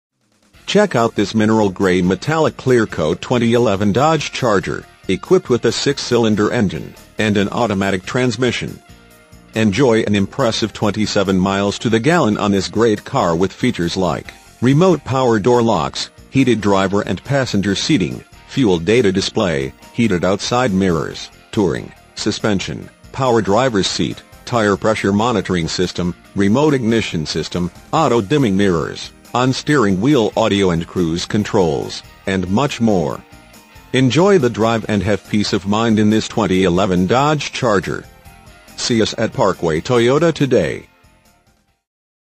2011 Dodge Charger Parkway Toyota Englewood Cliffs, NJ 07632
Dodge Charger Imagine driving this Mineral Gray Metallic Clear Coat Dodge Charger 4 Dr Sedan, equipped with a 6 Cylinder engine and an automatic transmission. Enjoy an impressive 27 miles to the gallon on this great car with features like Remote Power Door Locks, Heated Driver and Passenger Seating, Fuel Data Display, Heated Outside Mirrors, Touring Suspension, Power Driver's Seat, Tire Pressure Monitoring System, Remote Ignition System, Auto-Dimming Mirrors, on steering wheel audio and cruise controls, and much more. Enjoy the drive and have peace of mind in this Dodge Charger. See us at Parkway Toyota today!
MILES: 39,373
VIN:2B3CL3CG8BH537648

Parkway Toyota
50 Sylvan Avenue
Englewood Cliffs, New Jersey 07632